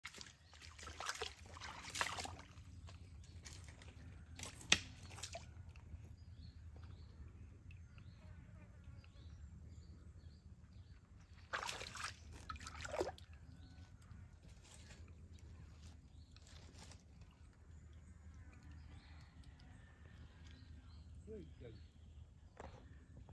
Komodo dragons eat carcasses of stingrays
Komodo eats the carcass of a stranded stingray and this Komodo does not want to eat it. Video shooting location on Komodo Island, Komodo Village